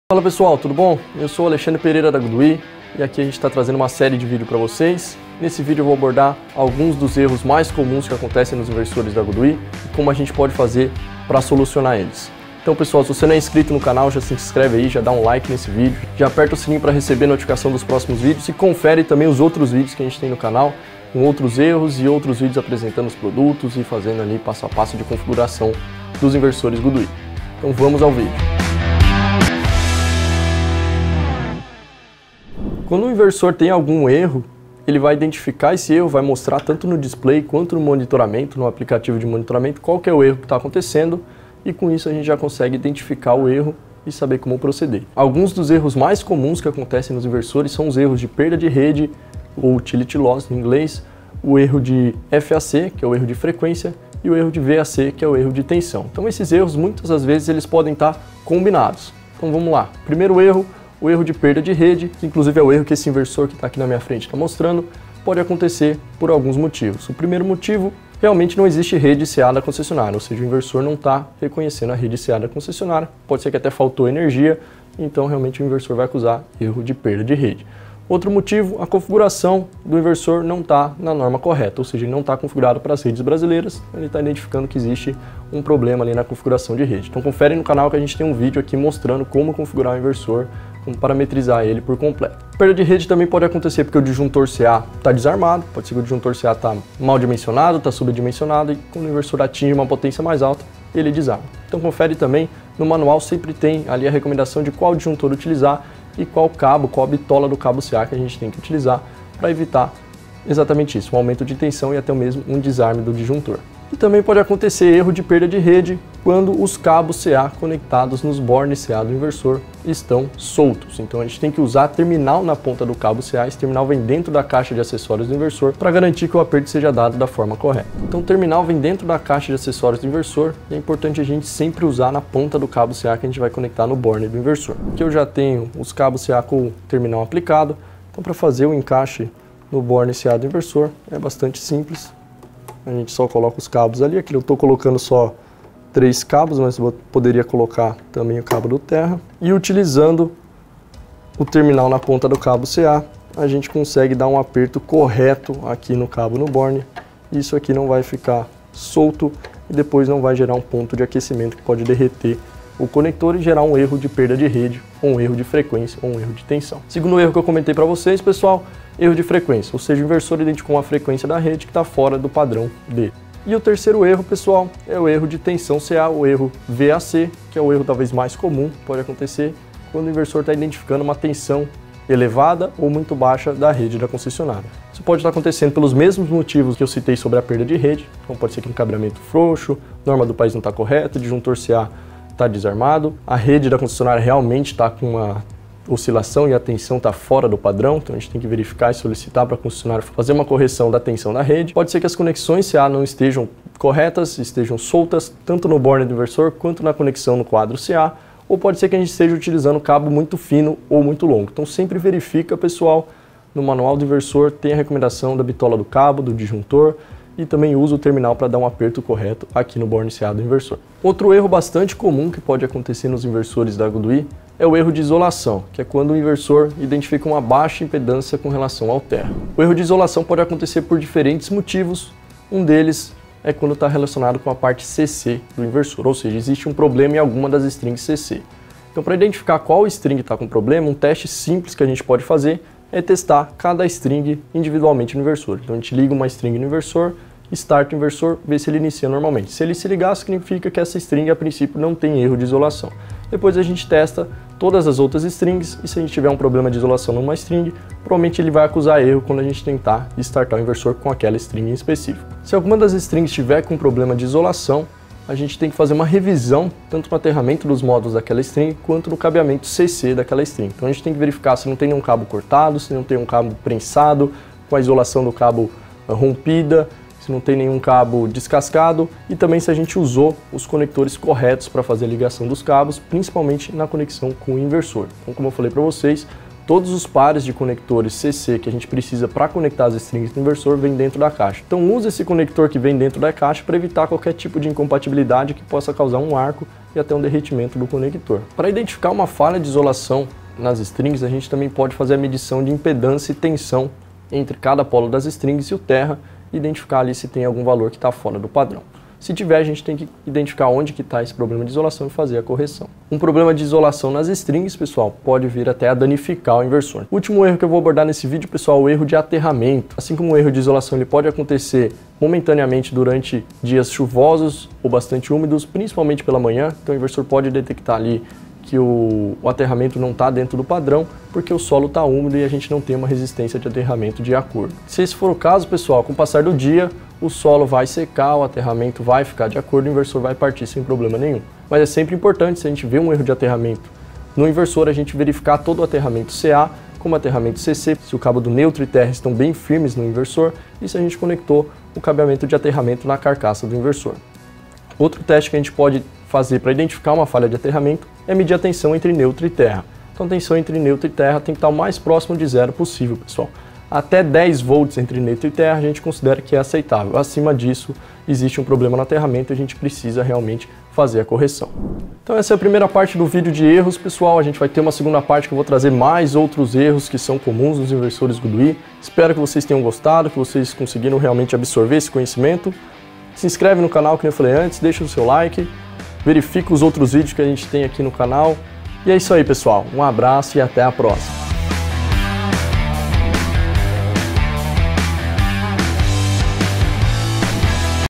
GoodWe - Erros comuns parte 1 (Perda de Rede, Fac, Vac, Isolação e Aterramento)
Principais erros que podem acontecer nos inversores GoodWe e quais as formas de identificar e solucionar cada um.